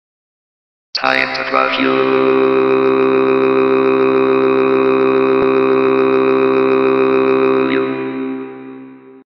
Insect-o-Vision
title: Bug Vision

scrushpt:
time to crush yoooooooooooooooooooooooooooooooooooooooooooooooooooooooooooooooooooooooooooooooooou

Offbeat Voices, English, Colossus.

Background, Animated, Anchors. Character, Symbols, Exclaimation! Symbol [yes that's really how they spelled it. Voki HQ [AKA Oddcast] is weird sometimes.]

FX: Ignite Shake + Ignite Insect Vision